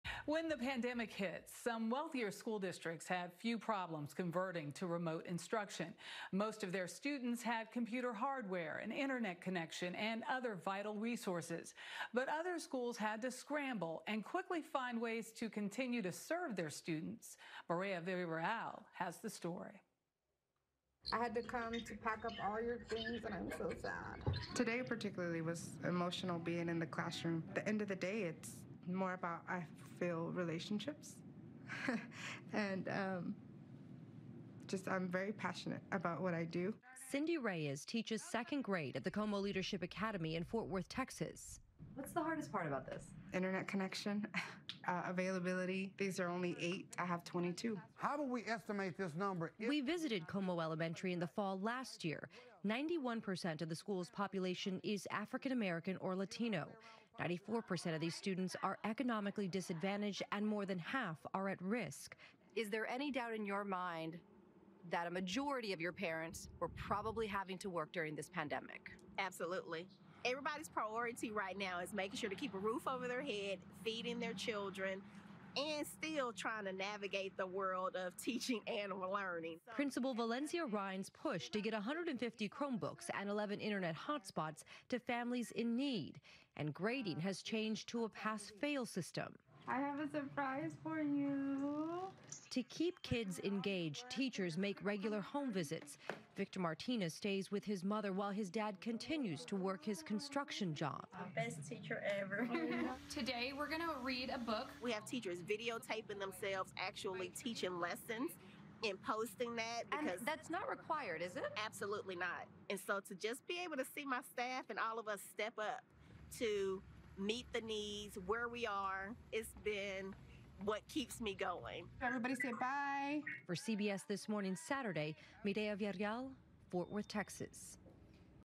Some school districts struggle with remote learning
When the coronavirus pandemic forced schools across the country to go digital, some wealthier school districts had few problems switching to remote learning. However, other areas where many students lack access to internet, computer hardware and other vital resources had to scramble to make the drastic conversion. Mireya Villarreal reports.